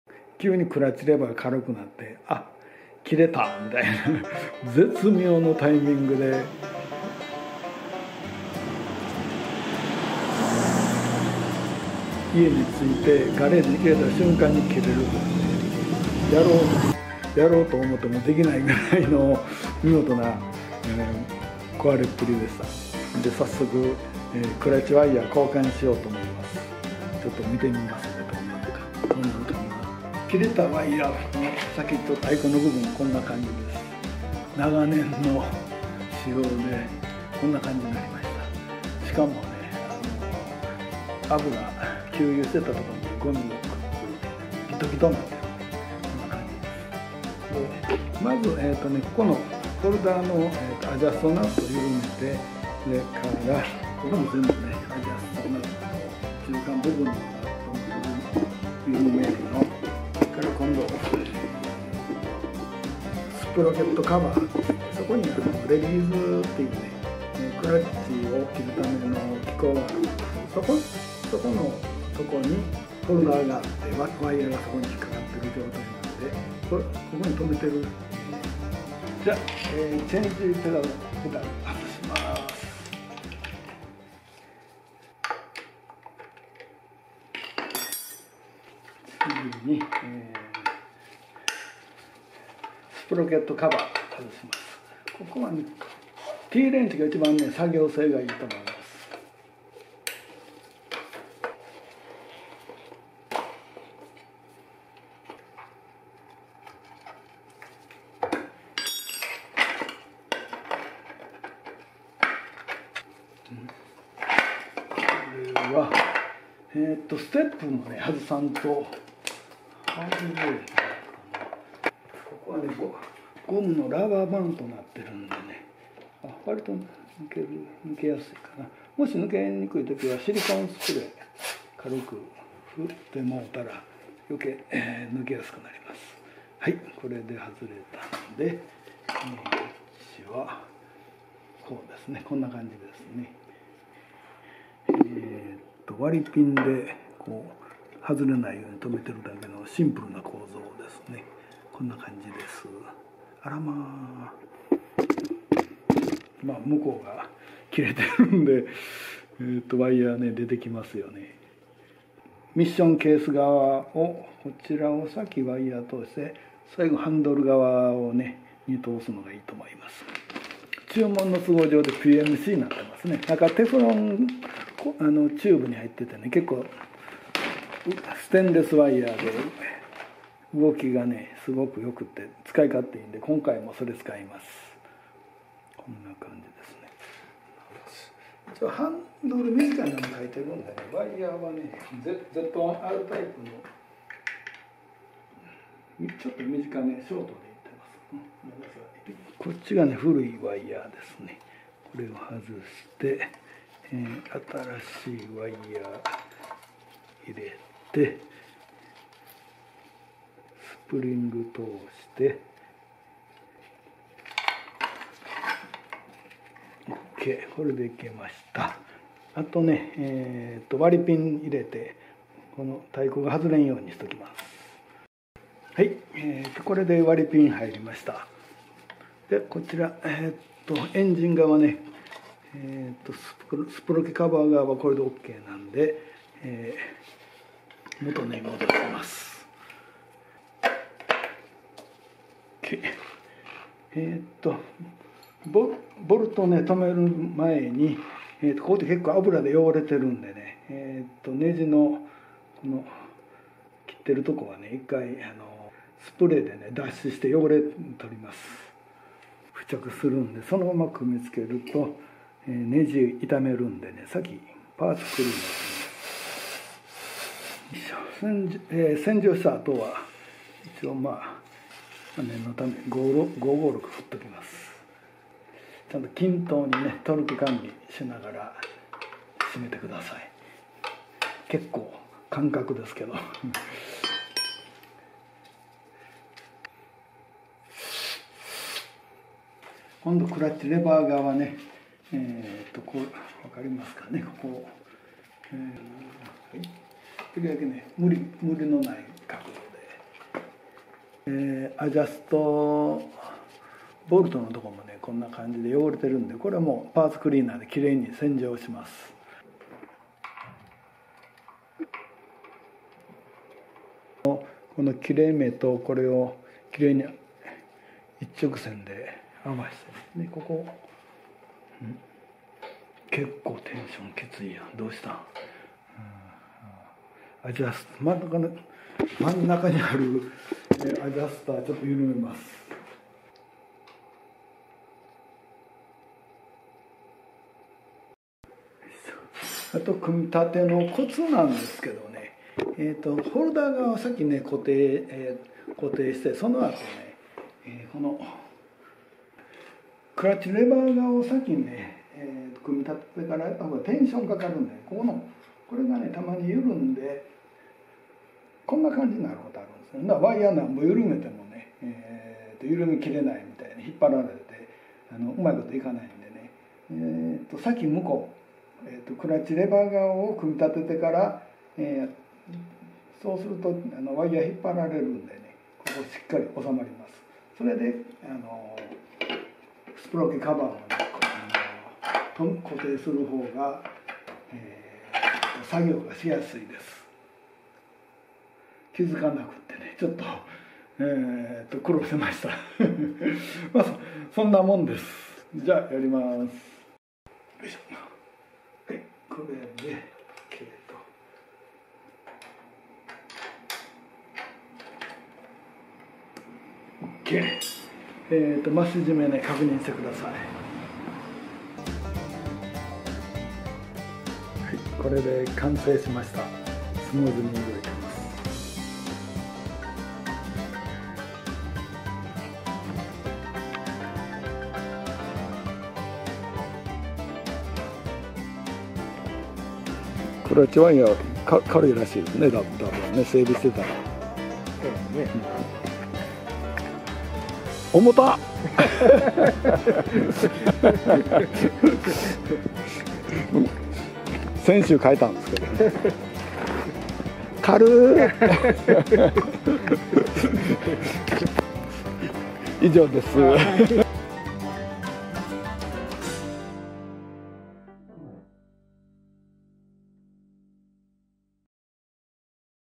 カワサキZ1/2系クラッチワイヤー交換 メンテ分かりやすく解説
旧車バイクのメンテナンス入門編です　知らず知らずのうちに経年劣化するクラッチワイヤー、動きが徐々に渋くなったり最悪断裂により走行不能に陥ります、新品ワイヤー交換するとスムーズな動きに感動すること請け合い　今回は分かりやすく動画で交換手順を解説しました。